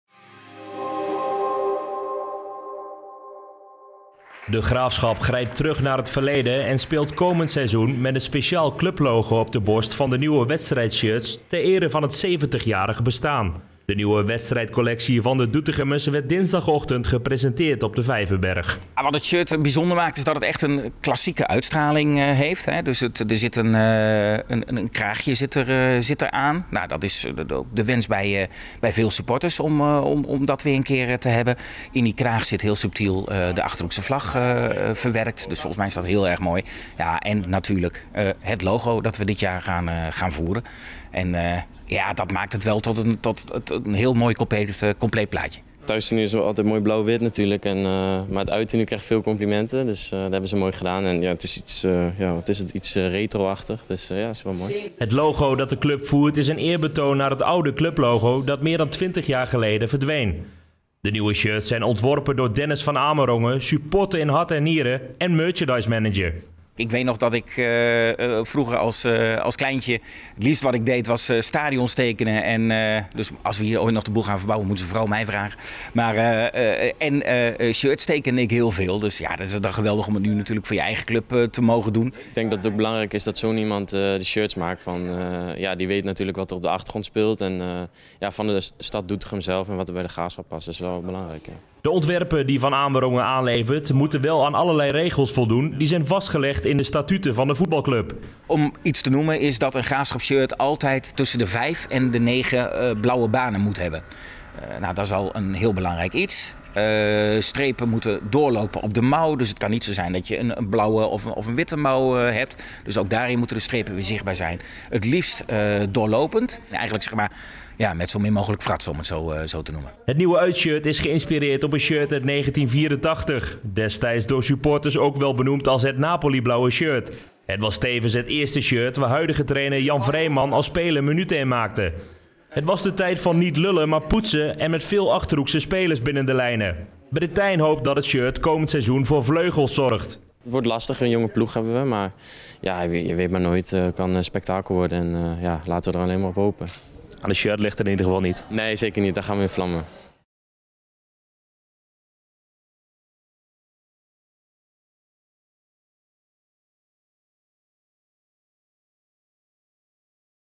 De Graafschap grijpt terug naar verleden voor nieuw wedstrijdshirt
De Graafschap grijpt terug naar het verleden en speelt komend seizoen met een speciaal clublogo op de borst van de nieuwe wedstrijdshirts, ter ere van het zeventigjarig bestaan. De nieuwe wedstrijdcollectie van de Doetinchemmers werd dinsdagochtend gepresenteerd op stadion De Vijverberg.